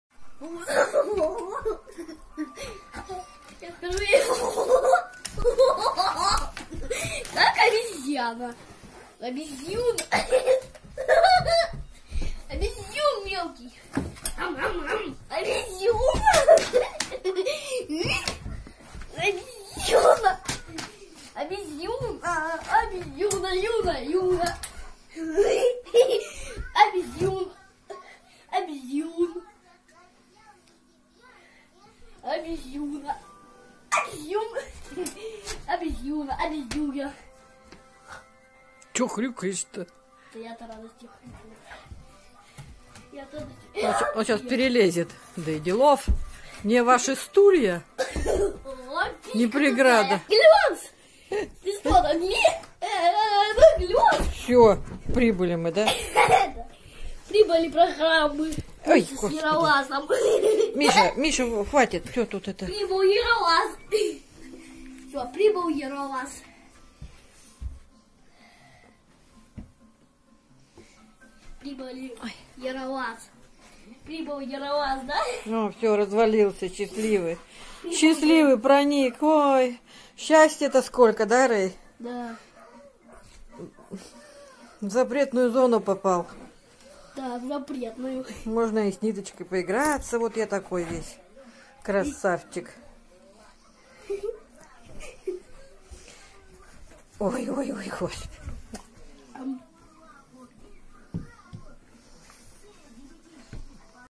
Немецкая овчарка, как котенок, играет с ниточкой.
На даче в деревне Миша и Рэй( немецкая овчарка) играют.